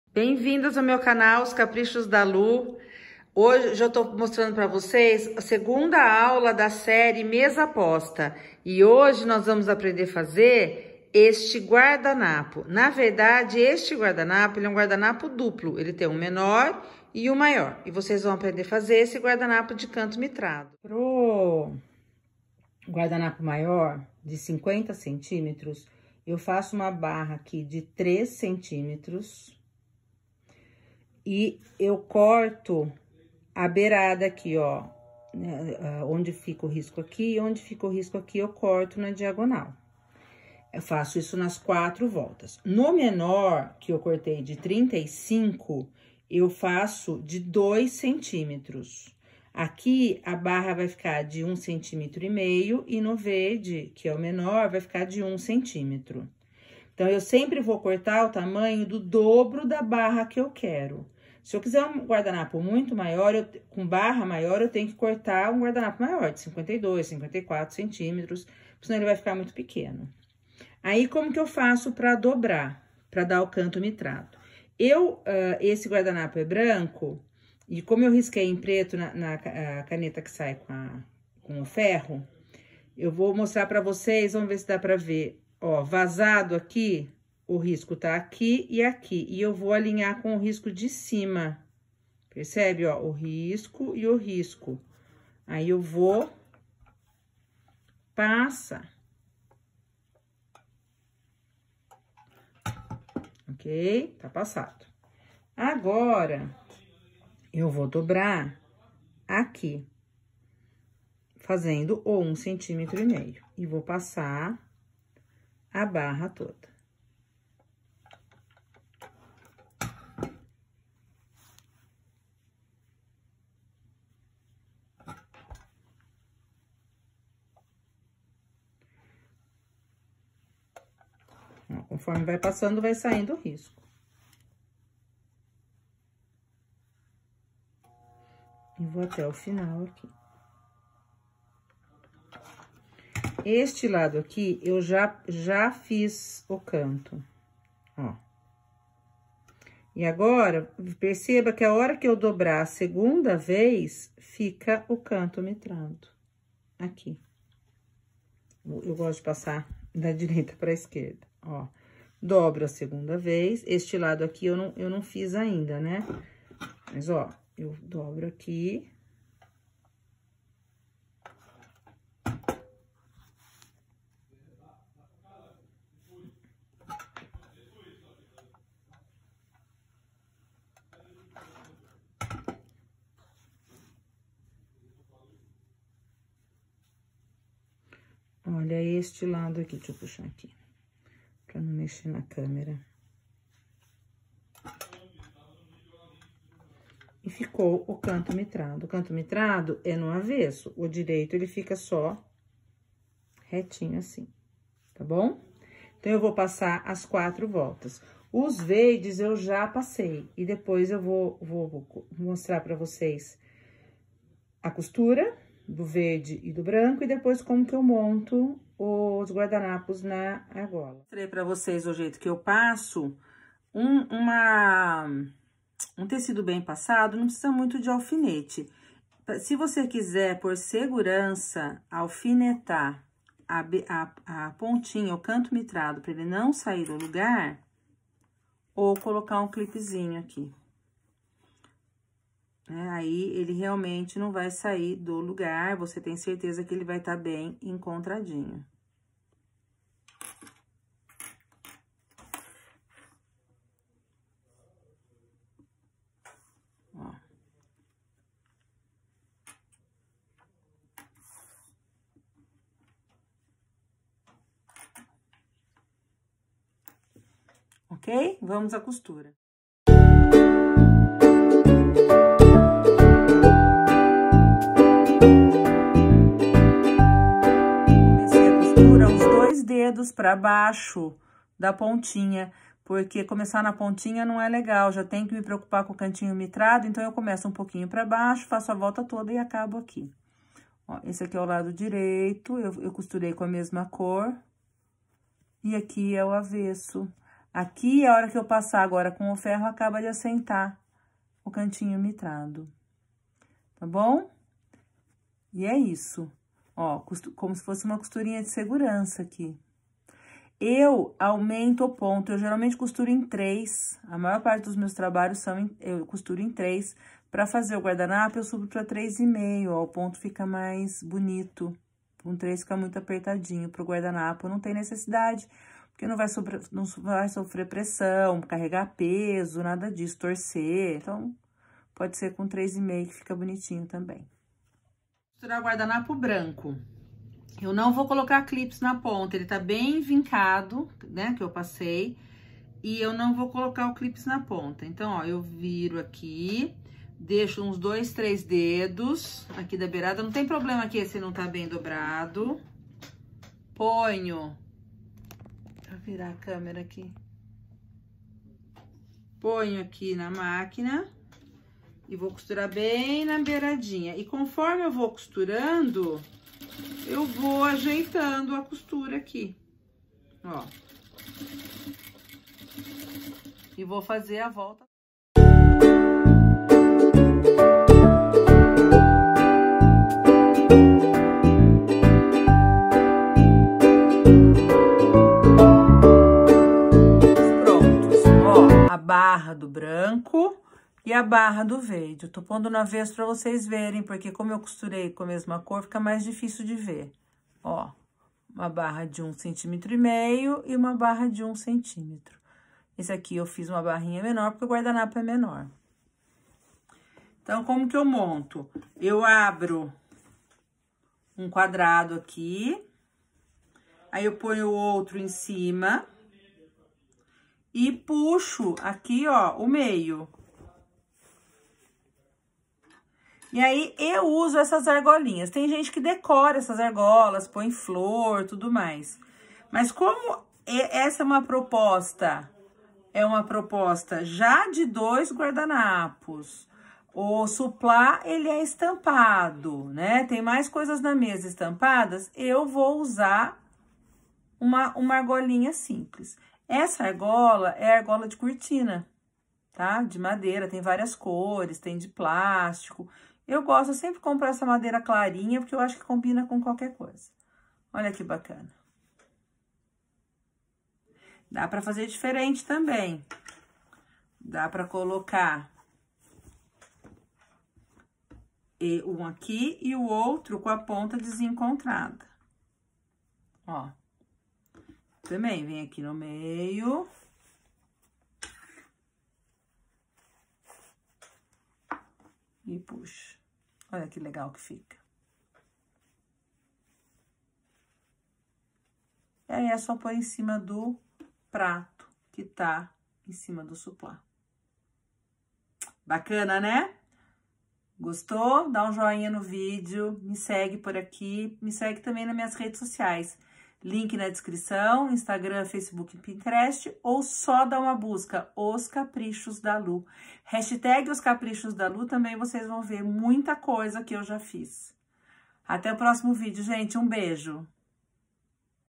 Guardanapo de tecido com canto mitrado - SÉRIE MESA POSTA - AULA 2
Aprenda a técnica do canto mitrado.